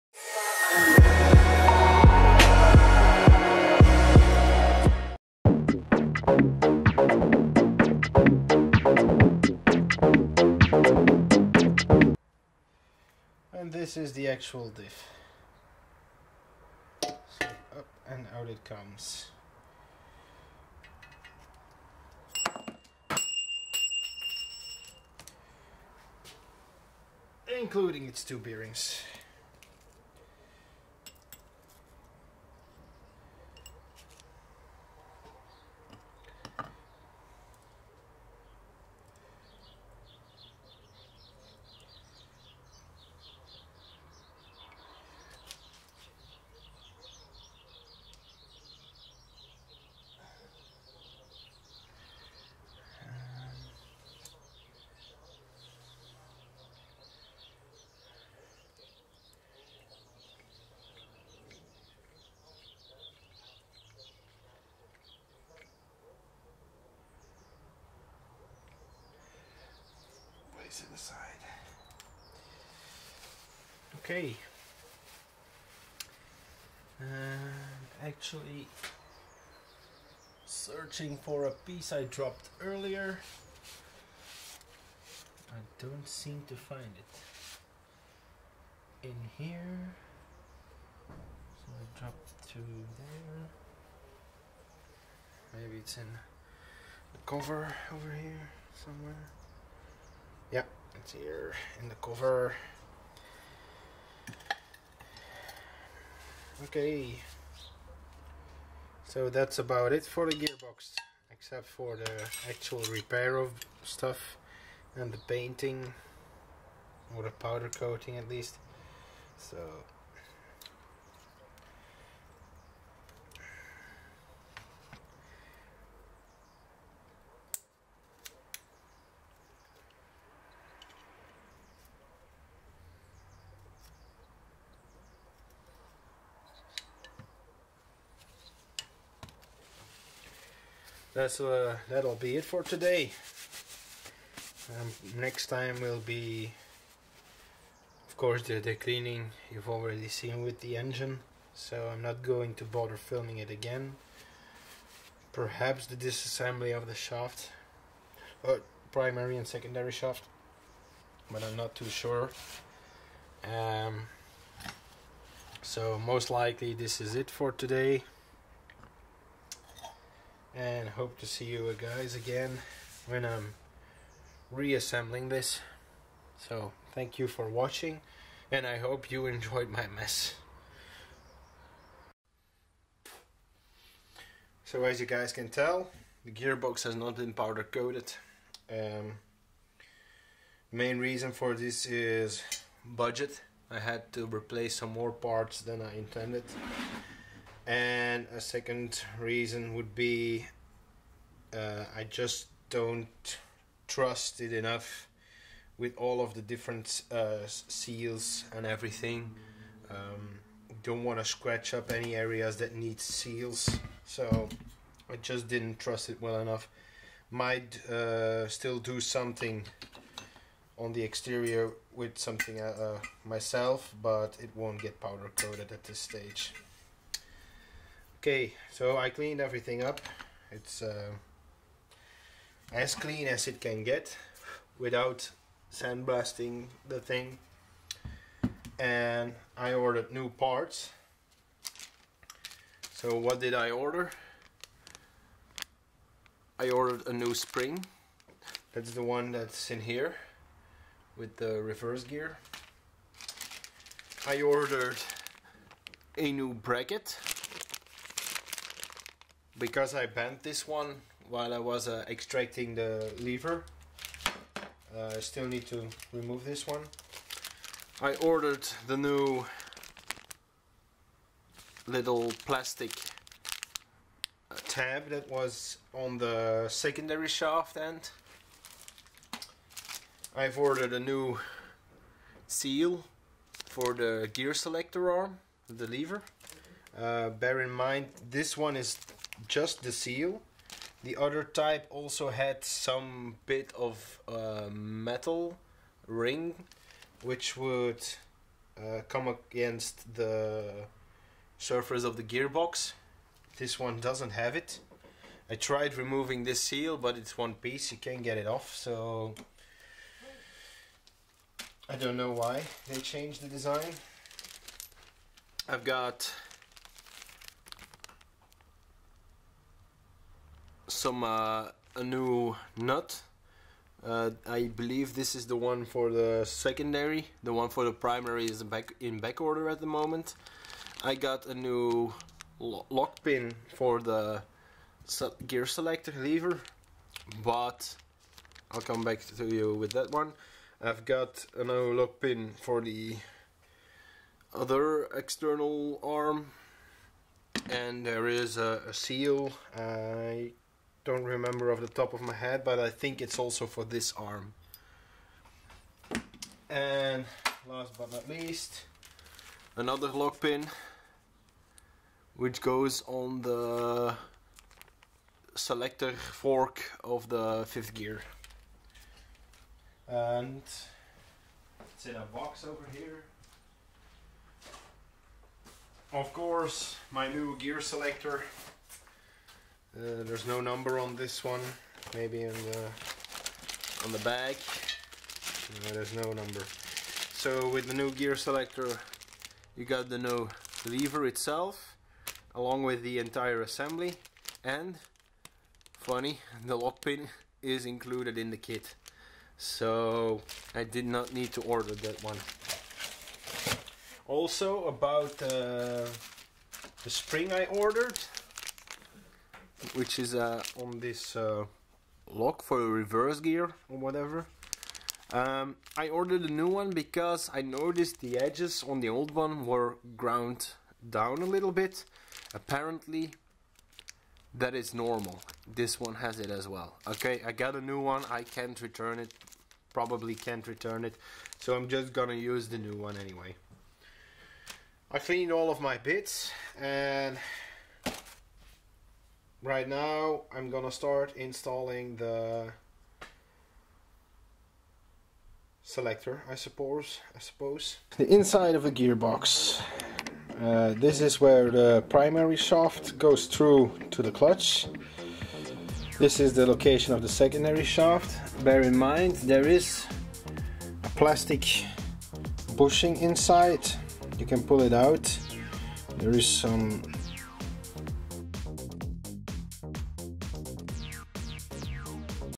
Peugeot 207 - BE4 Gearbox Overhaul (Part 6)
Ronke's DIY Workshop

In this series of videos I plan to record as much as possible of the process of overhauling a front wheel drive gearbox.

In this episode, the gearbox got completely stripped down and we will now start the overhaul...